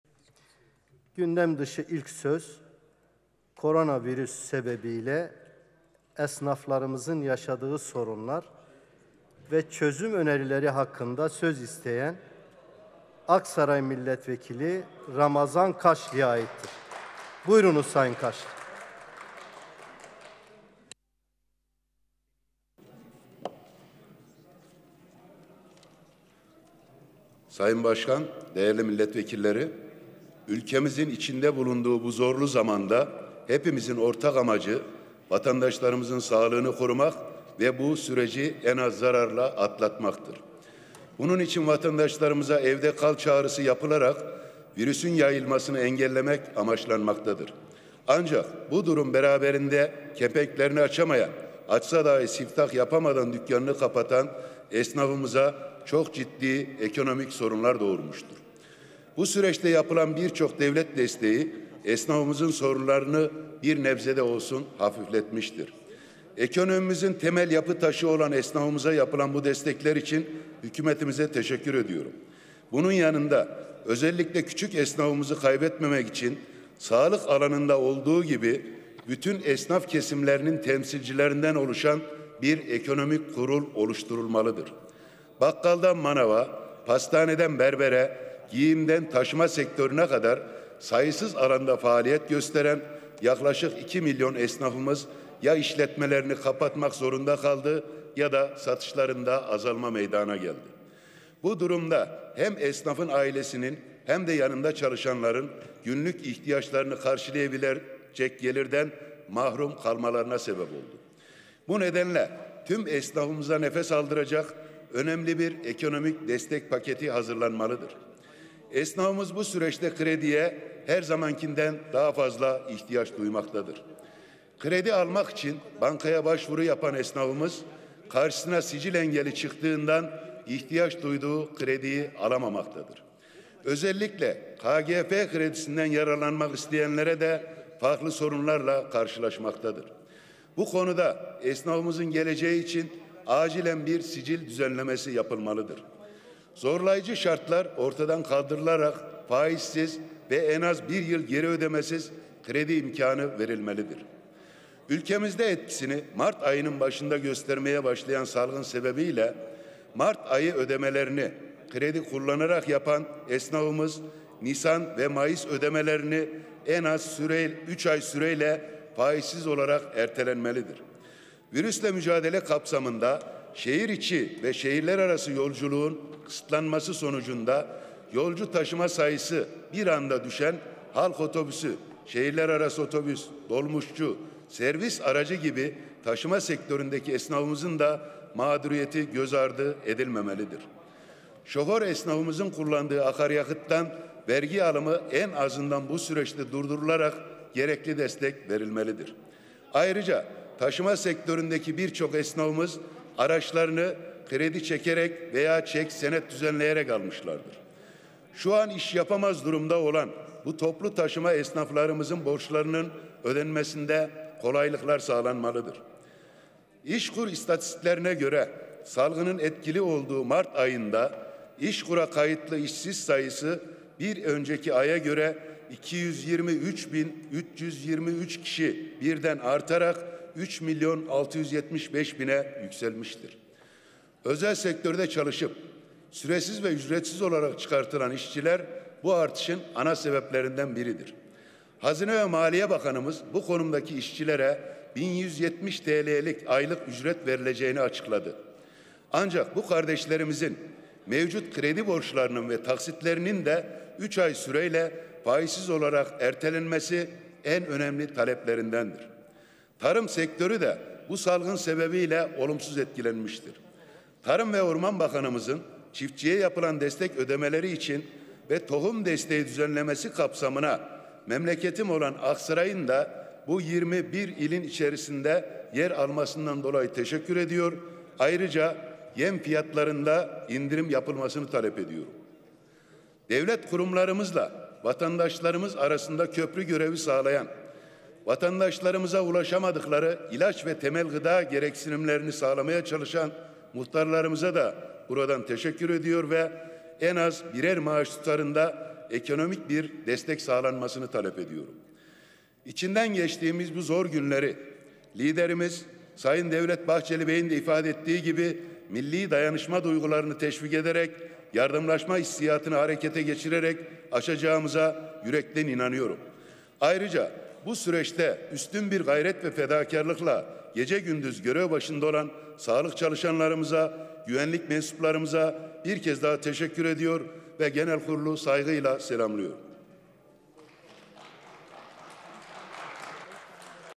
"Korona Virüsü Sebebiyle Esnaflarımızın Yaşadığı Sorunlar" Hakkında Gündem Dışı Konuşma
MHP Aksaray Milletvekili Ramazan Kaşlı, TBMM kürsüsünden Esnafımızın, Çiftçimizin ve İşçi/İşveren sorunlarını dile getirdi.  Ülkemizin içinde bulunduğu bu zorlu zamanda, uymamız gereken 'evde kal' çağrısını bir kez daha hatırlatan Sayın Kaşlı, yaşanılan ekonomik sorunlar hakkında çözüm önerilerinde bulundu.
İçinden geçtiğimiz bu zor günleri, liderimiz Sayın Devlet Bahçeli Bey'in de ifade ettiği gibi milli dayanışma duygularını teşvik ederek, yardımlaşma hissiyatını harekete geçirerek aşacağımıza yürekten inandığını belirten Sayın Kaşlı ayrıca bu süreçte, üstün bir gayret ve fedakârlıkla gece gündüz görev başında olan sağlık çalışanlarımıza, güvenlik mensuplarımıza teşekkür etti.